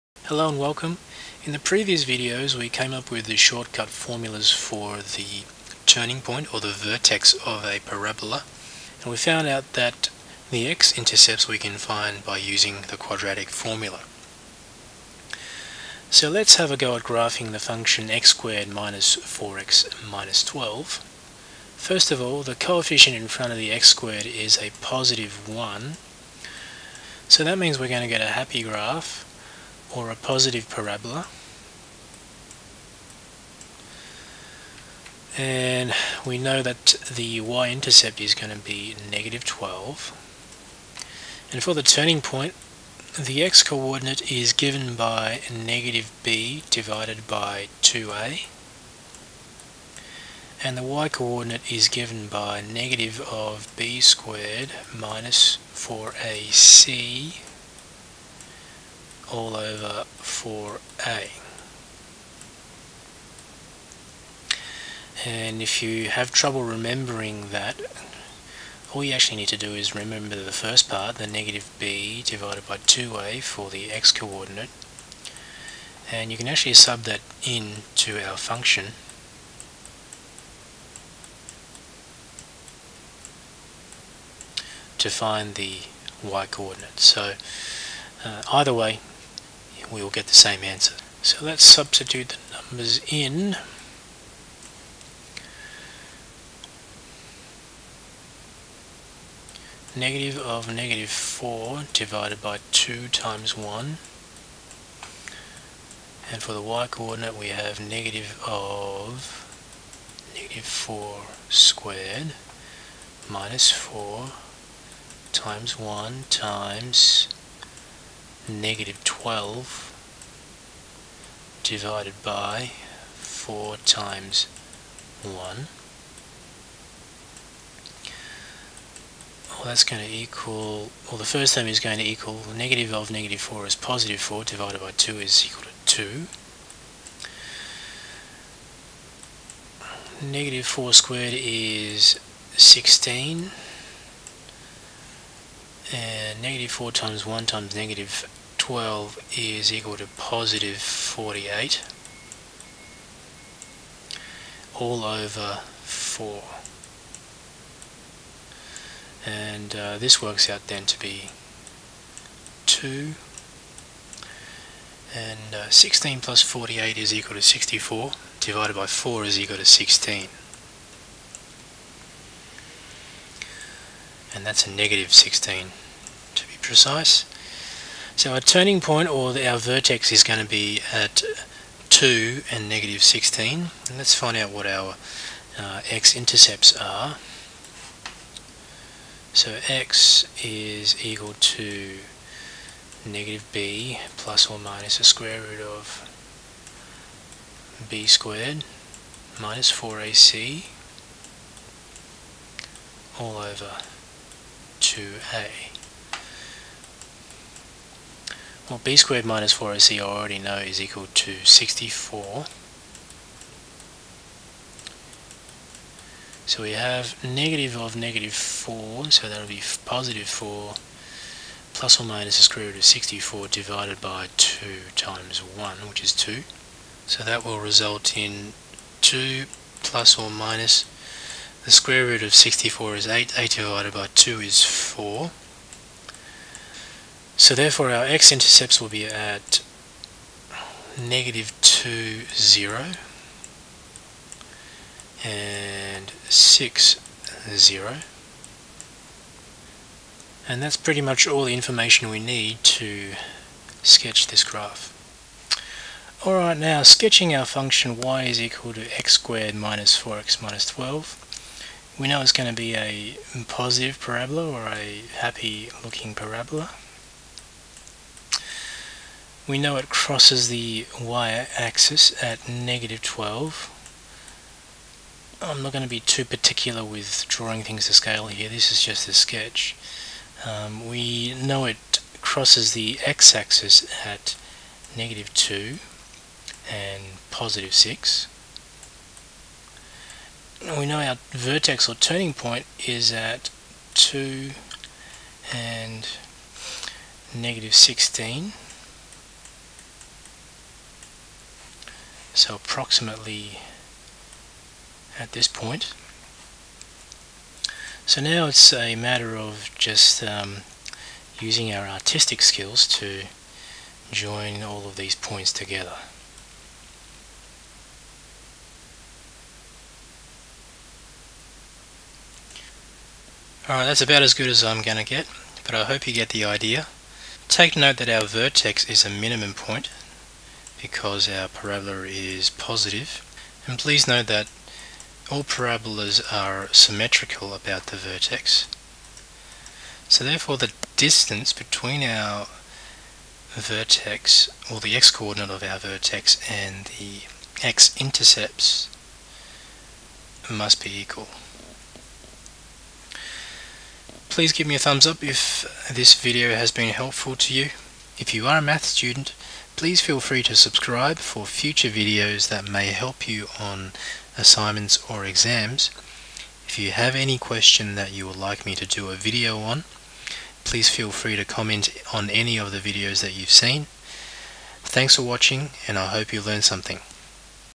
How to Sketch a Parabola - Example 2 (y = x^2 - 4x - 12)
In this video, we graph the trinomial / quadratic function y = x^2 - 4x - 12 by finding its concavity, y-intercept, using the turning point formulas and the quadratic formula for the x - intercepts. Remember, there are only 4 pieces of information you need to accurately sketch a parabola:

1. Concavity - determined by whether the coefficient in front of the x^2 term is positive or negative
2. y-intercept - the value of the trailing free term
3. Vertex or turning point - given by (-b/2a, -(b2-4ac)/4a)
4. x-intercepts, if there are any - determined by the quadratic formula